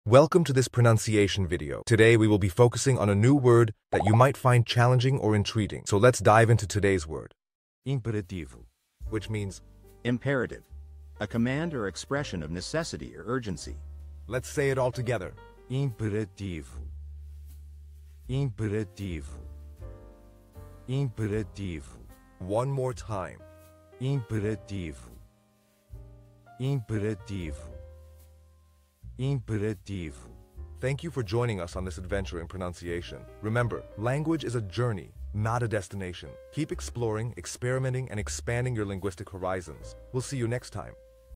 How to pronounce Imperativo
Master the Pronunciation of 'Imperativo WHICH MEANS IMPERATIVE' - which means : Imperative: The Command or Expression of Isso or Urgency. 📝 with @PortuguesePronunciation 🎓🗣️ - [Your Guide to Portuguese]

Learn to pronounce 'Imperativo' perfectly in Portuguese with our easy and engaging tutorial by @PortuguesePronunciation! 📖📽️ In this comprehensive video, we provide you with native speaker examples 🗣️👂 and phonetic breakdowns 🎵 to ensure you get the pronunciation just right.

How to say Imperative in Portuguese